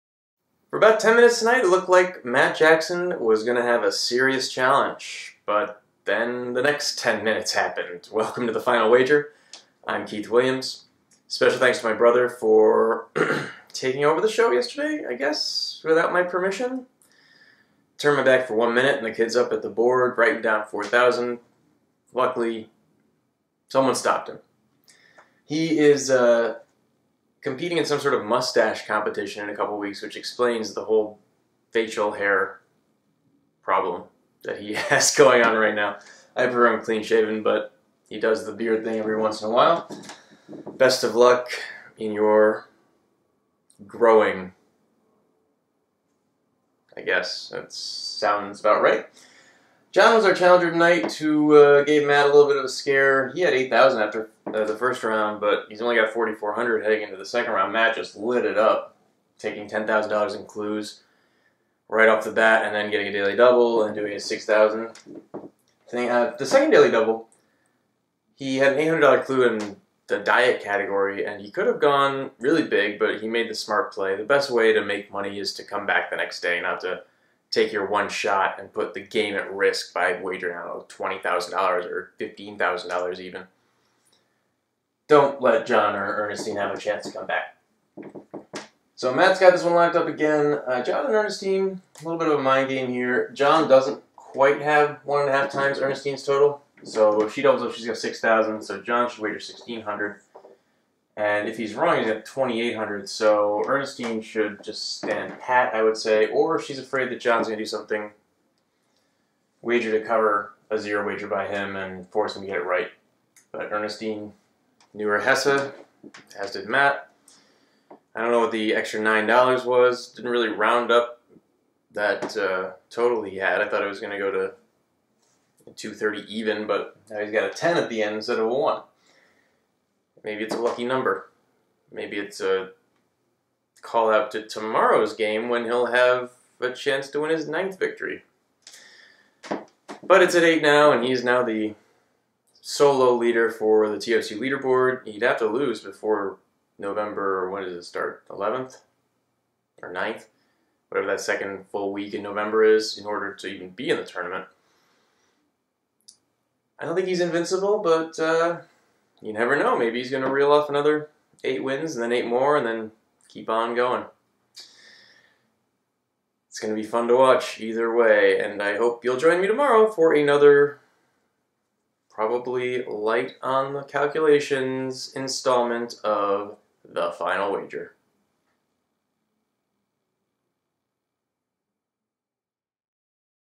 The Final Wager – Tuesday, October 6, 2015
2003 Jeopardy! College Champion Keith Williams breaks down the wagering for the match Matt Jackson vs. John Kozempel vs. Ernestine Gardner, which first aired October 6, 2015.